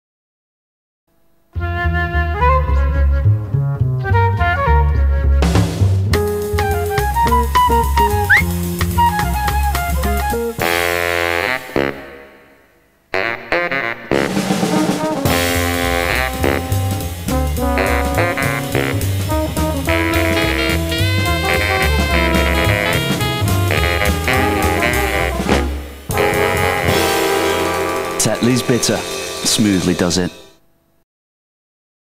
Tetley's Bitter Beach Advert On TNT UK TV
Advert for Tetley's Bitter broadcast on TNT in the UK.
Probably broadcast in 1999.